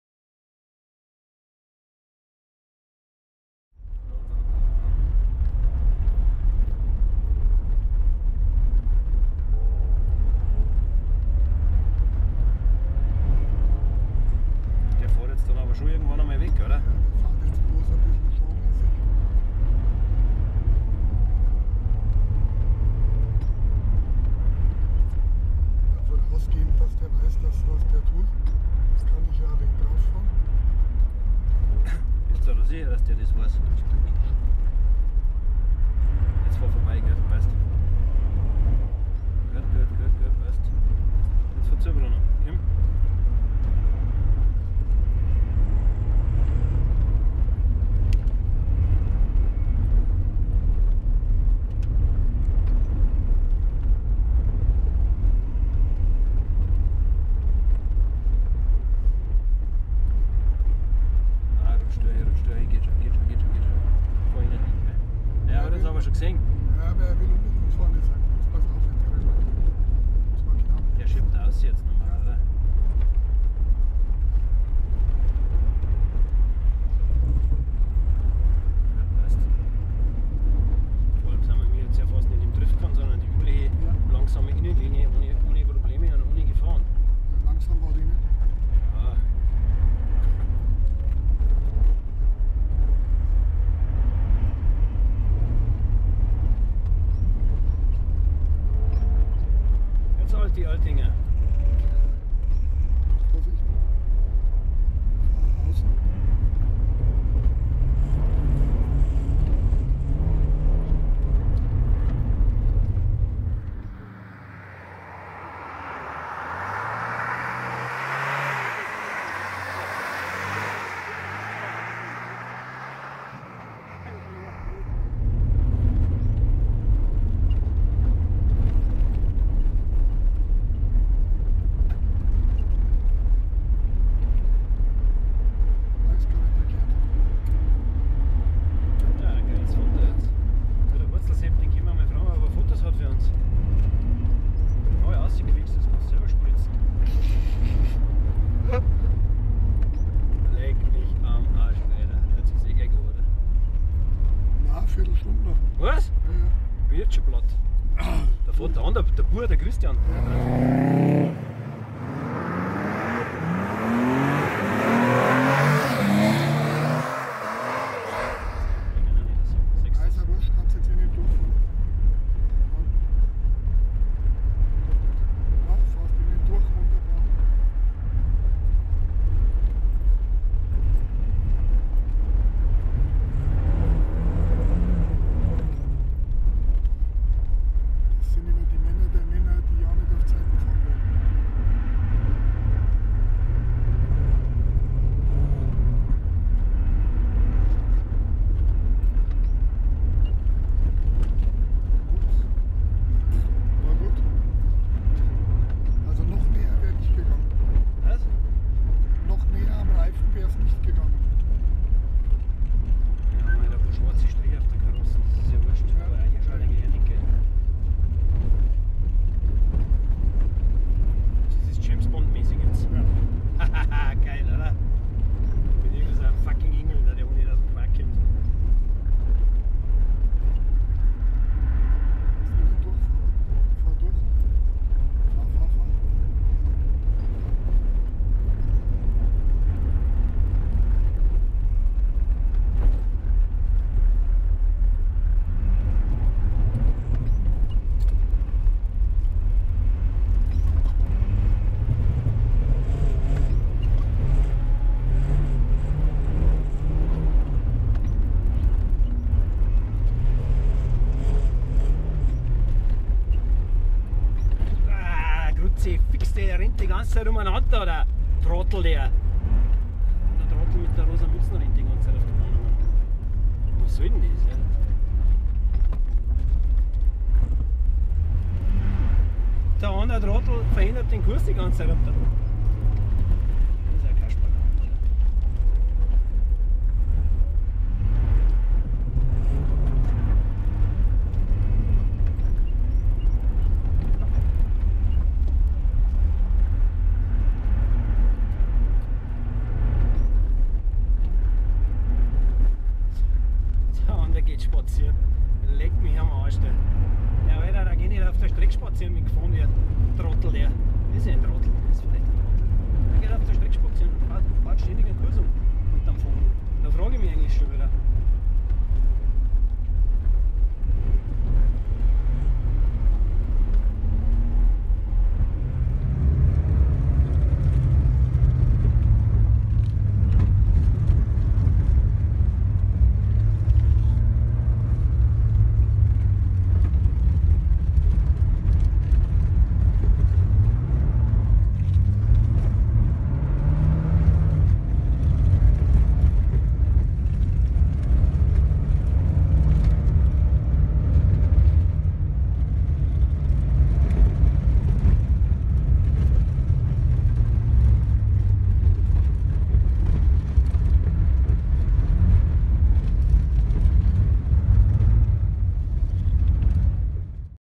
Winterfahrtraining auf Eis im Thomatal 2019 onboard
Freies Fahren im Thomatal
Porsche 924S
Fahrer Christian Sternecker, Tom Schwarzmeier